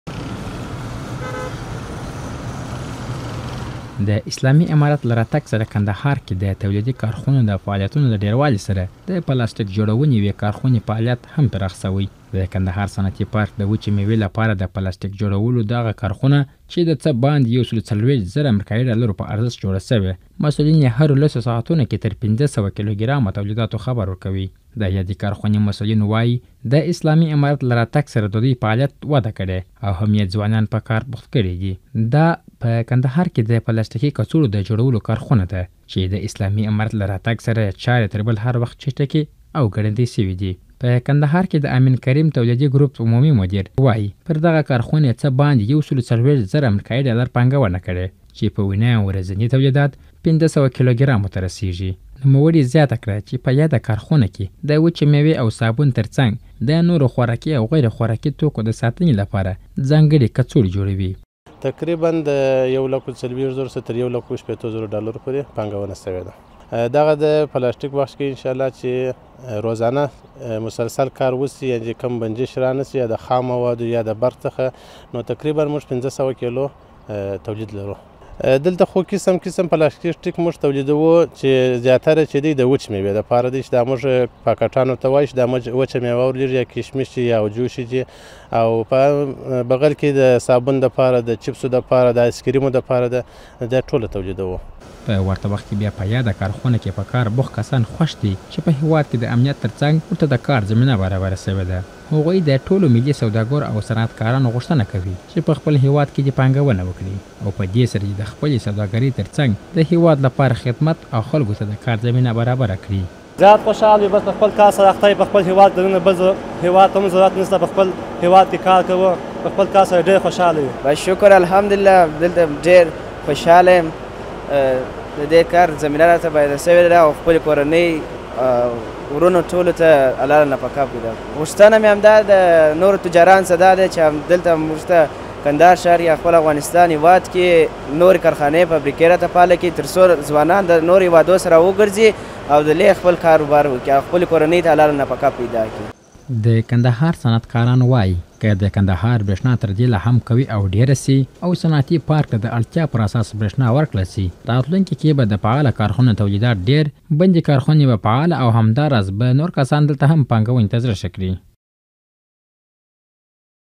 کندهار کې د پلاستیکي خلتو د تولیداتو کچه زیاته شوې
کندهار کې د پلاستیکي خلتو کارخونې مسولین وایي، چې دا.اپه واکمنېدو سره یې د تولیداتو کچه زیاته شوې او په ورځني ډول ۵۰۰ کیلو ګرامه پلاستیکي خلتې تولیدوي.